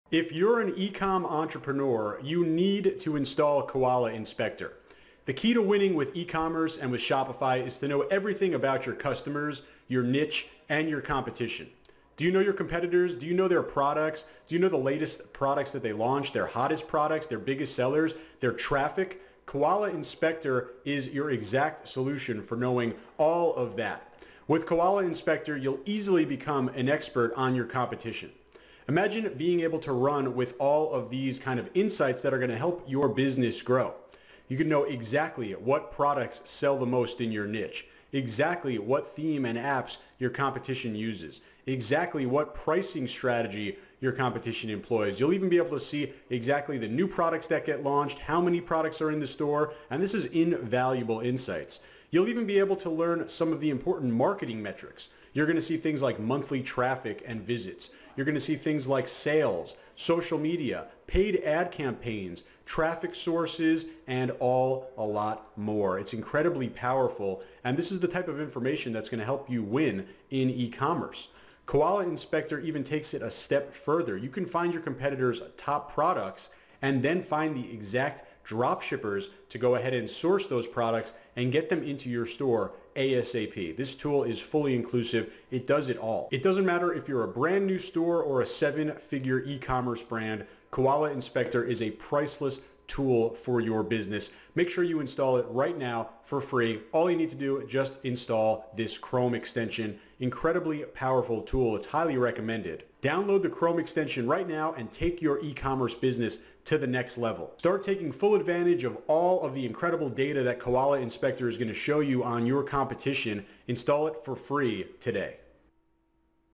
Koala Inspector - Shopify Spy & eCommerce Spy Tool
Take the guesswork out of your Shopify & eCommerce strategy and win the Dropshipping game. Understand your niche, find winning products, avoid costly mistakes, and apply strategies that work.

*How to Spy on Shopify Competitors (And Why You Should Do It!)